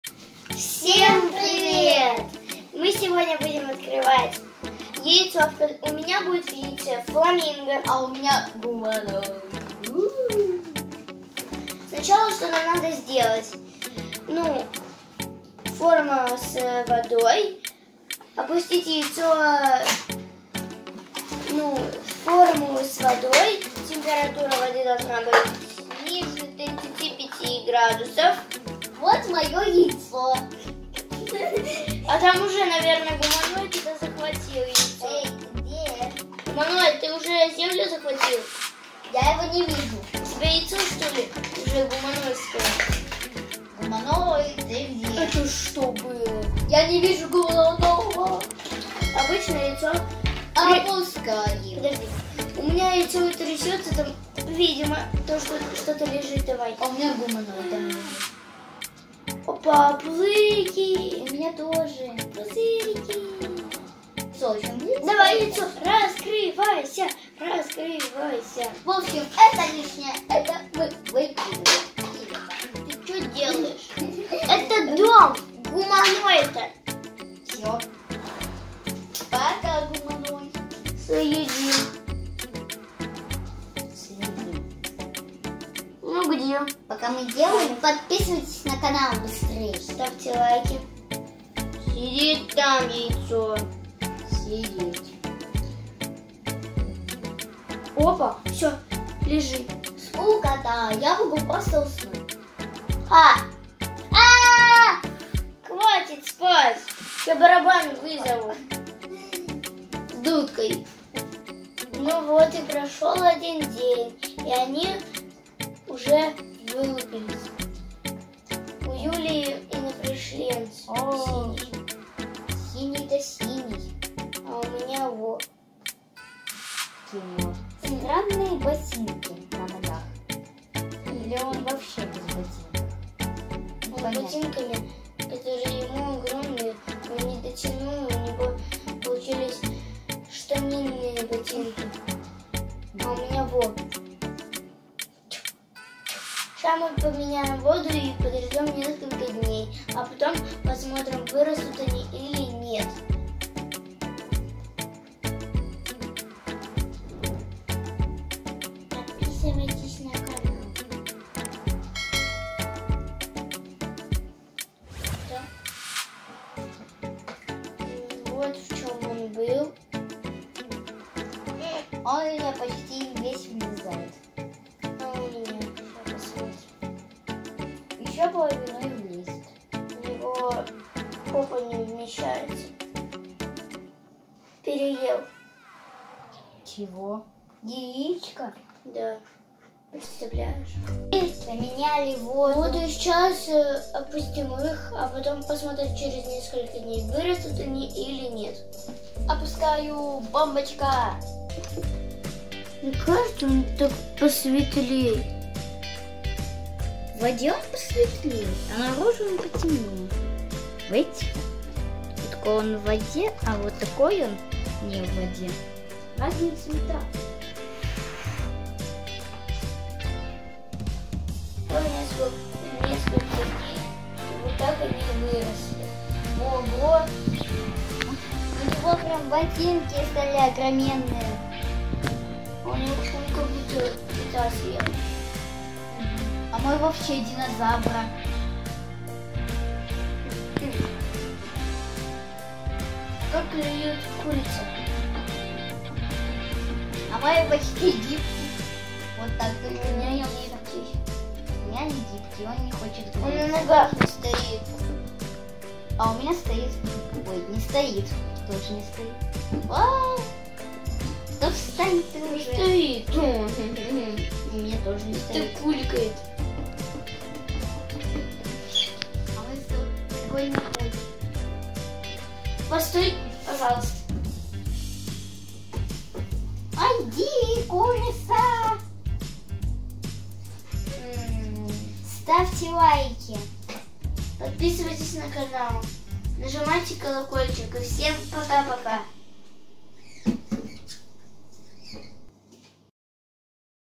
Как вырастить игрушку из яйца /Фламинго из яйца/ Attivio Яйцо растущее/ для детей
Яйцо опускаем в воду и из него вылупляется животное, а потом животное растет.